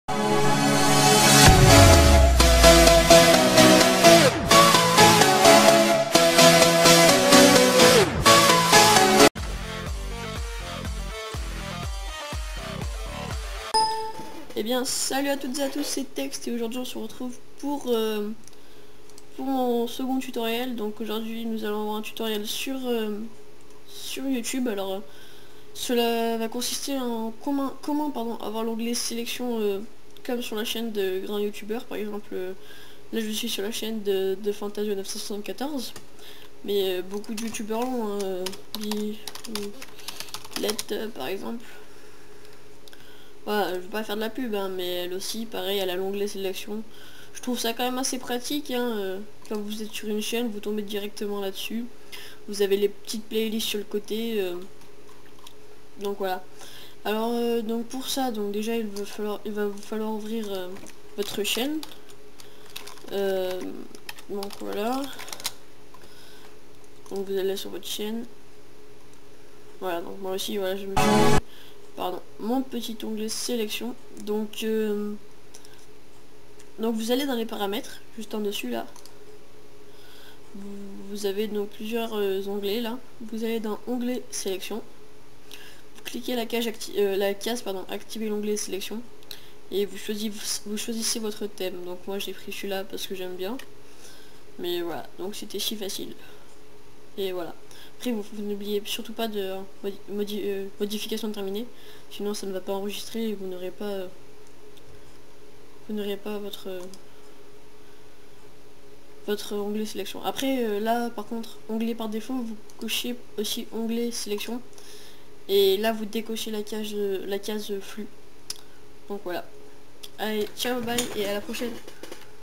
[TuTo] comment avoir l'onglet sélection sur YouTube
Lis cette description Stp

Voici le tuto pour avoir l'onglet sélection sur YouTube comme les grand youtubeur comme diablox9 ,Biloulette , TheFantaSio974 et bien d'autre . Pour ce il vous suffit d'aller dans "paramètres" puis "onglet sélection " cocher la case "activer l'onglet sélection " ensuite aller dans l'onglet "info et paramètres " de sélectionner onglet par défaut sélection de décocher "toujours dirigé les abonnées vers l'onglet flux " et voila quand quelqu'un vas sur vôtre chaîne il tombe ensuite a vous de personnalité comme vous voulez have fun =)